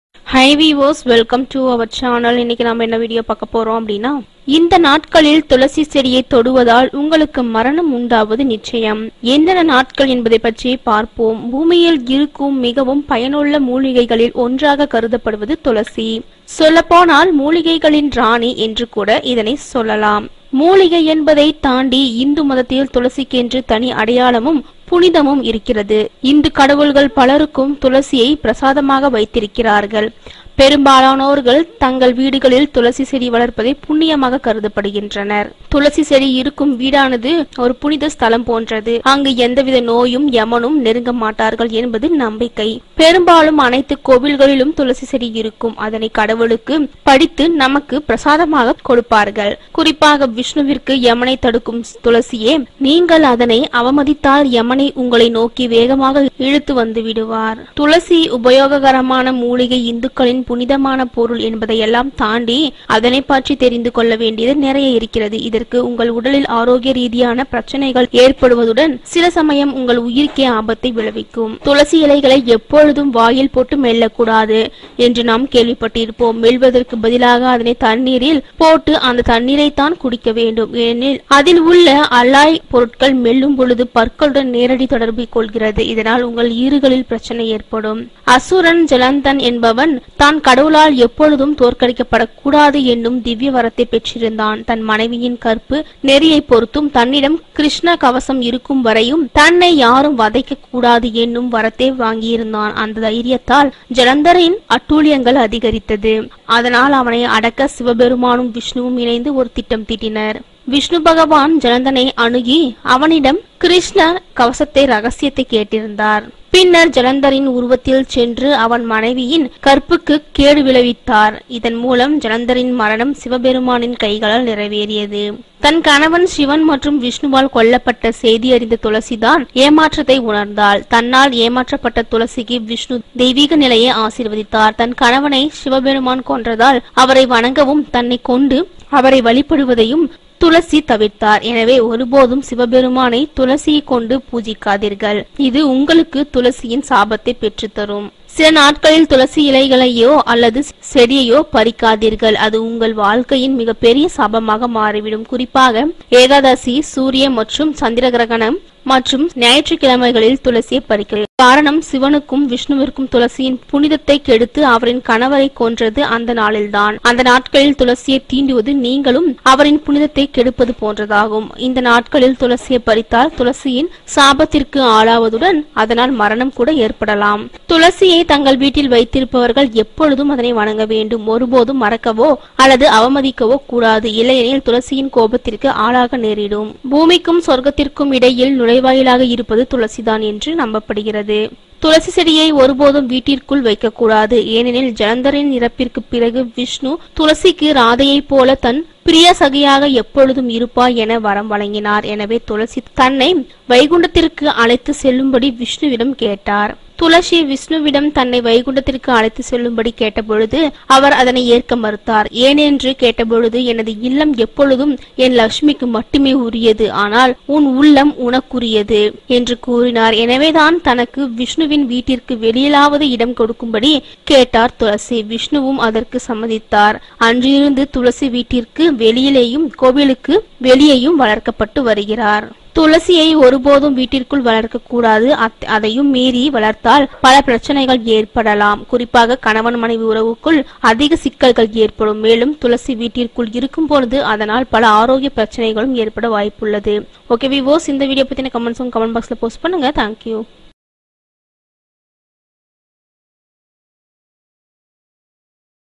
இந்த நாட்களில் துளசி செடியை தொட்டால் என்ன நடக்கும்தெரியுமா ..?
இந்த நாட்களில் துளசி செடியை தொட்டால் மரணம் நிச்சயம்